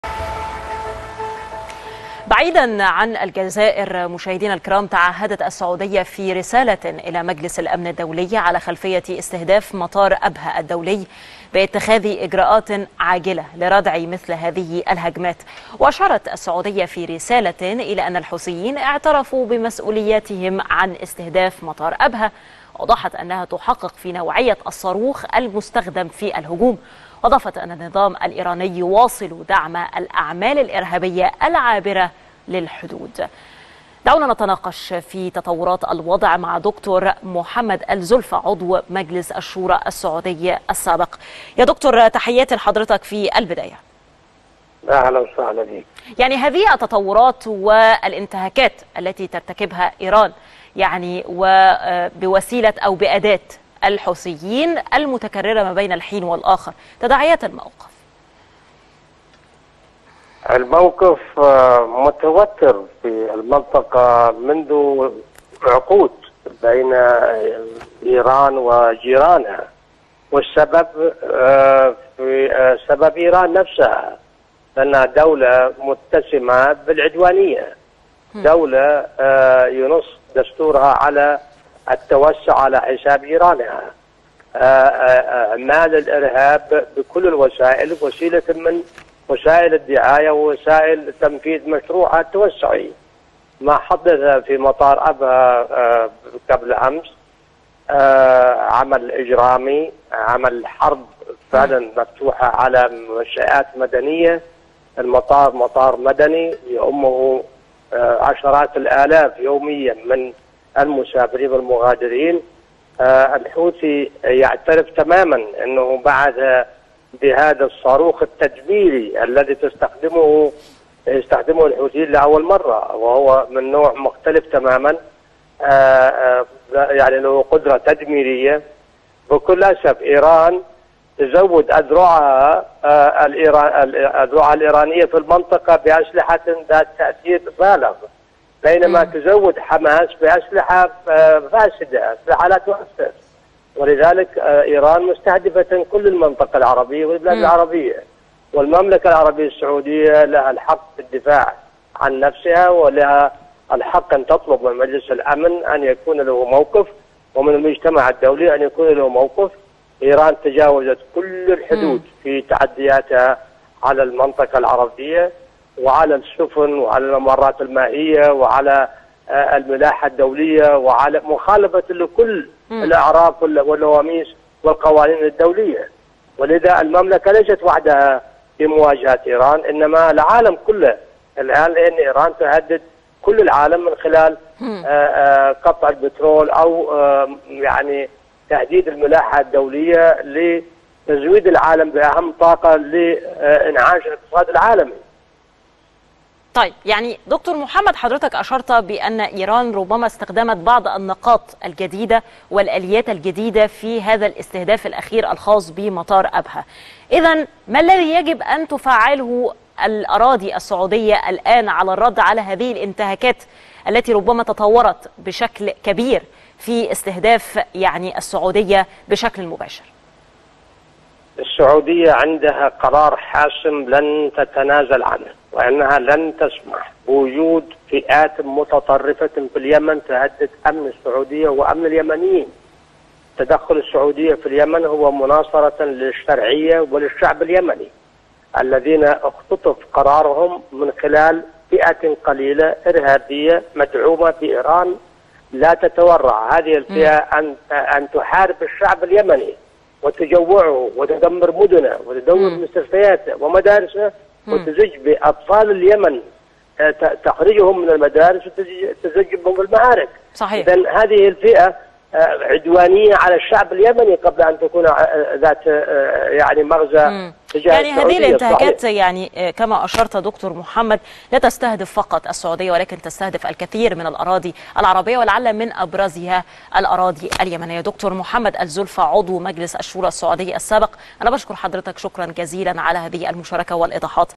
الآن | السعودية تتعهد باتخاذ إجراءات عاجلة للردع عقب استهداف مطار أبها
الآن | السعودية تتعهد باتخاذ إجراءات عاجلة للردع عقب استهداف مطار أبها   | 15 يونيو 2019